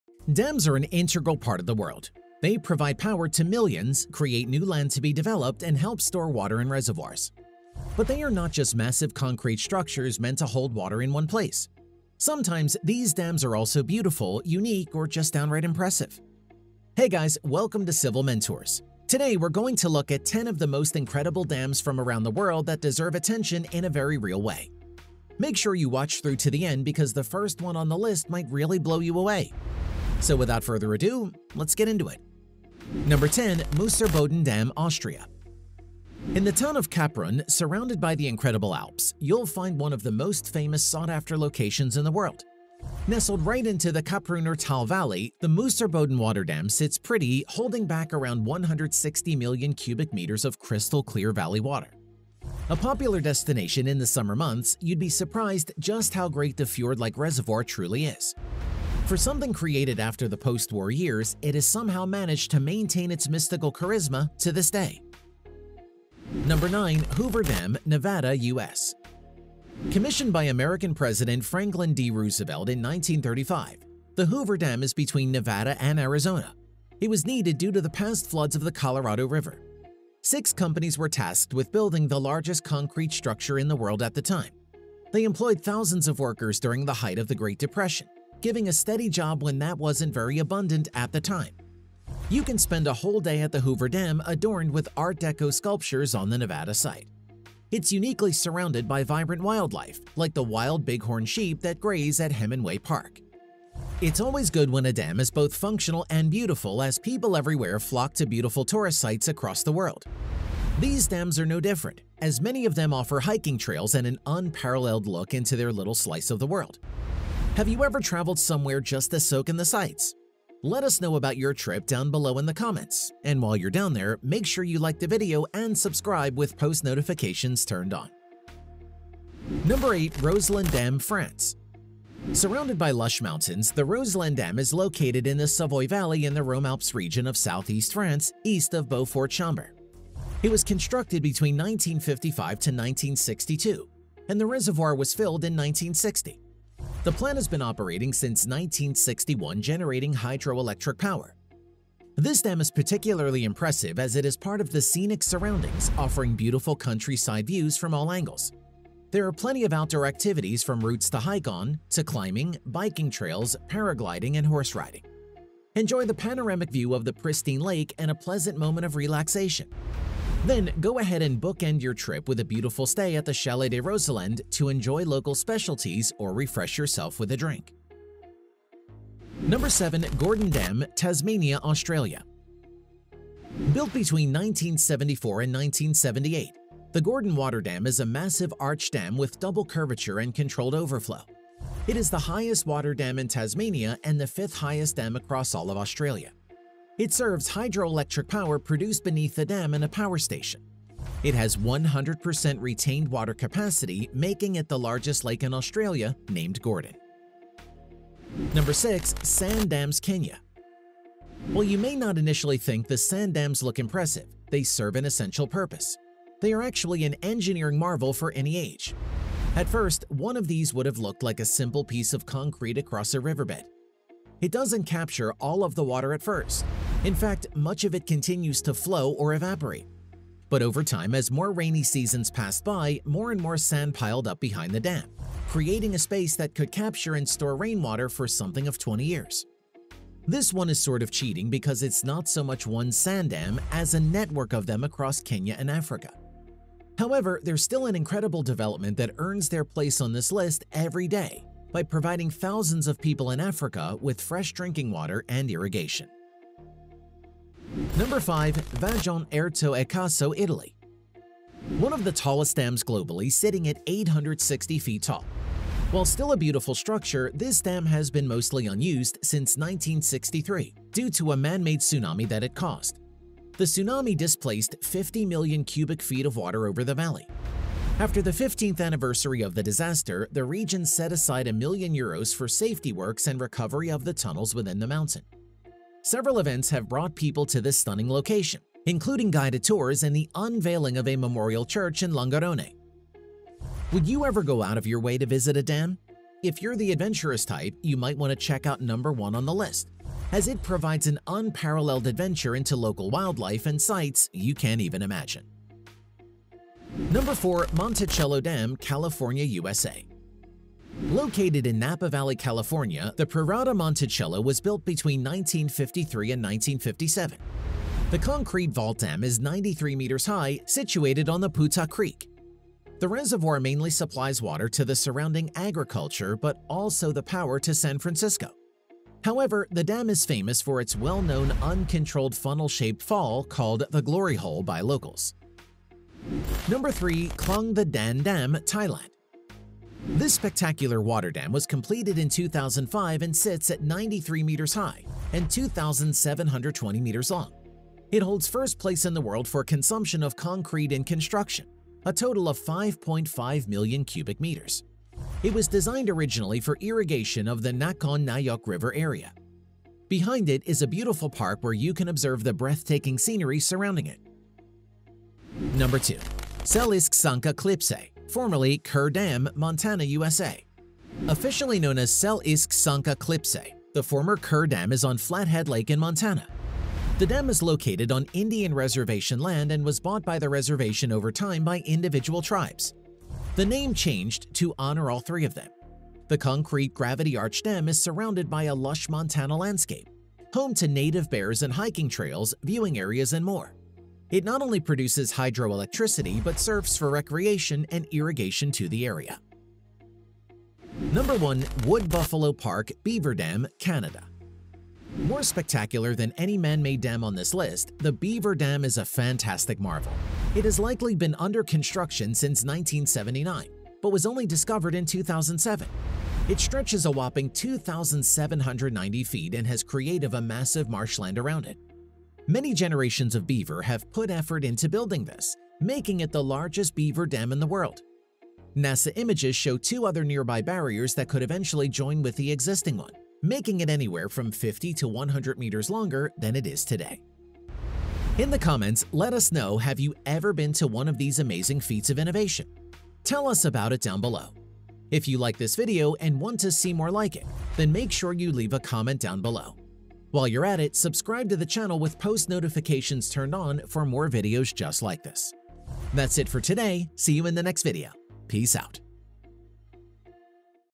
10 Most Incredible Dams in the World
There are many jaw-dropping structural feats around the world, each with its own interesting engineering designs.

But the one type of structure that always leaves us in great awe is dams.

Their sheer dimension and immense capacity tend to have a humbling effect on anyone who comes across these structures. It often feels a bit like standing in front of a calm, reassuring giant staring at you in all its glory.

And it goes without saying, dams are some of the most intricately designed structures in the world

Here are 10 most incredible dams in the world.